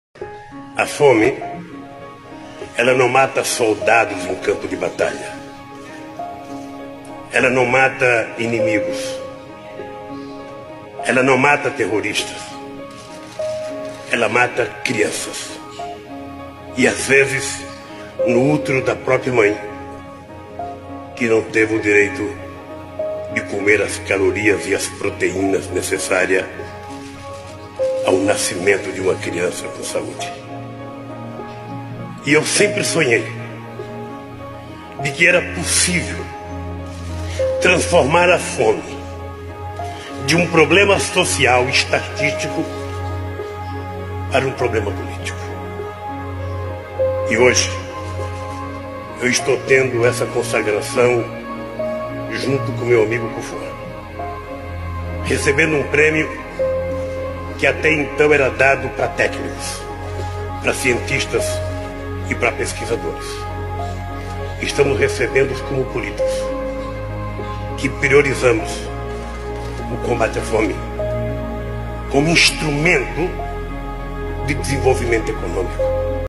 Lula: A fome mata Criança!
Lula fala sobre o programa de combate a fome quando recebeu o prêmio Medalha de Ouro "Aliança Internacional Contra a Fome", do Fundo das Nações Unidas contra a Fome.